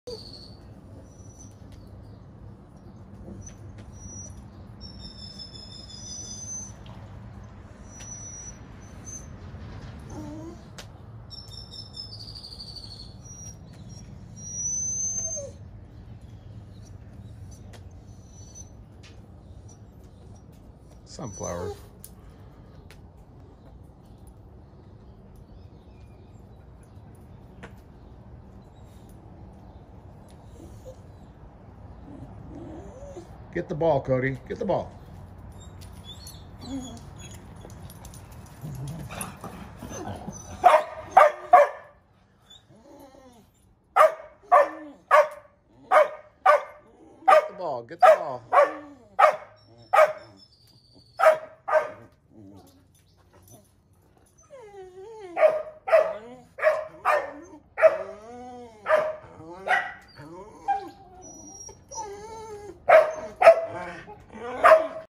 My ACD’s Blue heeler
Never a dull moment with these dogs.   10yr old dark female and 3yr old brother and sister.